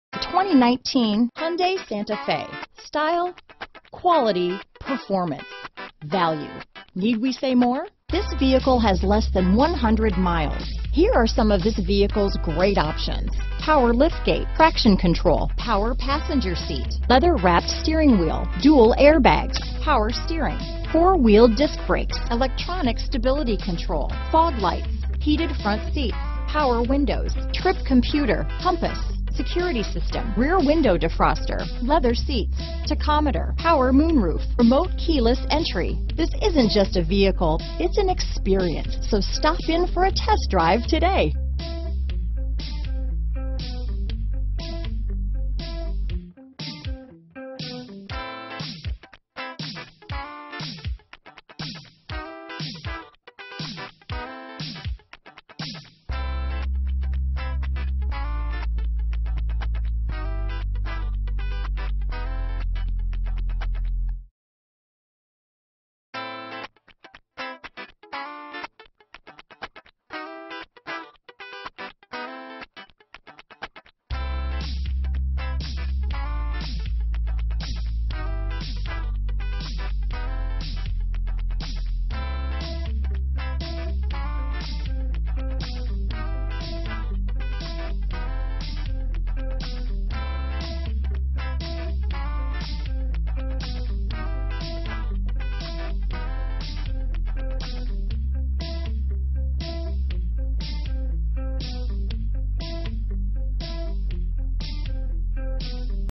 2019 Hyundai Santa Fe at Oxmoor Hyundai | Louisville & Lexington, KY H11836
Scarlet Red N 2019 Hyundai Santa Fe available in Louisville, Kentucky at Oxmoor Hyundai. Servicing the Lexington, Elizabethtown, KY, New Albany, IN, Jeffersonville, IN area.

Oxmoor Hyundai
8107 Shelbyville Rd

2019 Hyundai Santa Fe Limited 2.4 AWD, Beige Leather. 21/27 City/Highway MPGbrbrAll Rebates included in pricing. Some buyers will not qualify for all rebates. Price includes: $2,500 - Retail Bonus Cash. Exp. 02/28/2019, $500 - Regional Event Cash. Exp. 02/28/2019